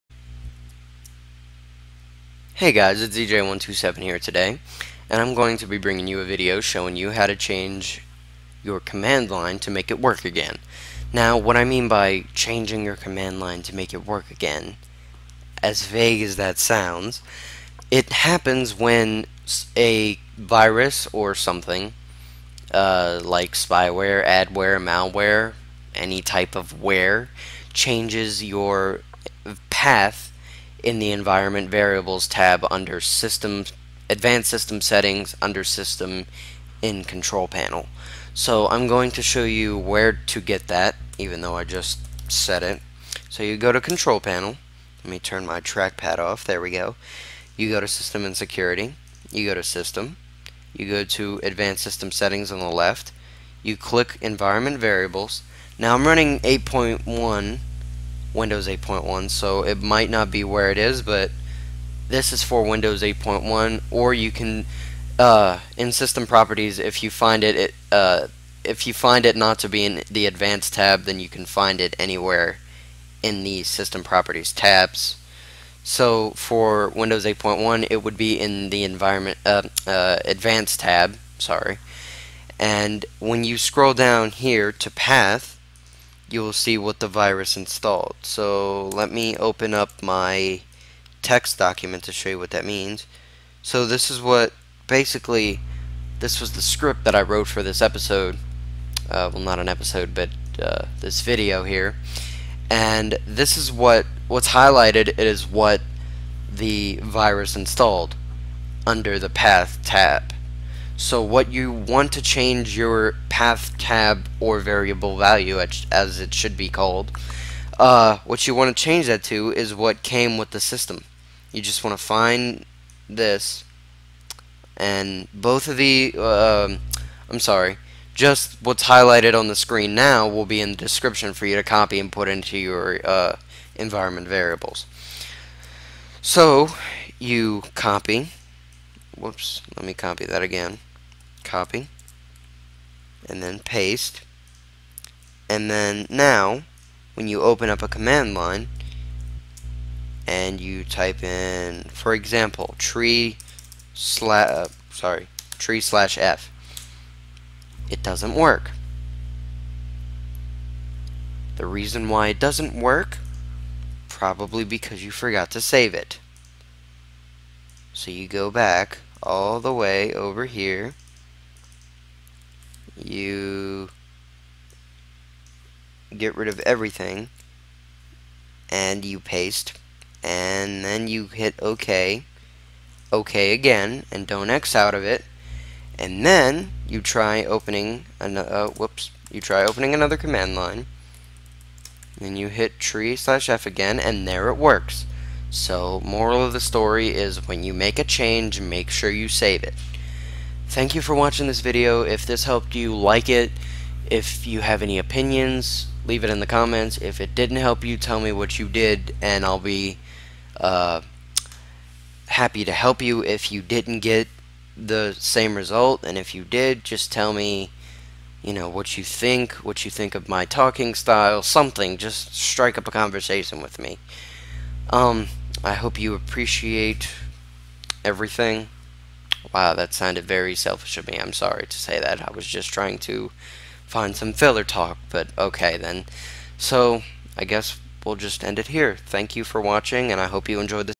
CMD Error fix
Hey guys, hope this video helps. Likie and comment please if you want some help. And sub for fresh content!

Edit: By my own stupidity, it wasn't a virus. FFmpeg is a FLV to MP4 encoder I downloaded, used once, and forgot about. I saw the QuickTime line and thought "Oh I don't remember installing any new QT software" :|

Copy this into Path's Variable value tab: %systemroot%\system32;%systemroot%;%systemroot%\system32\wbem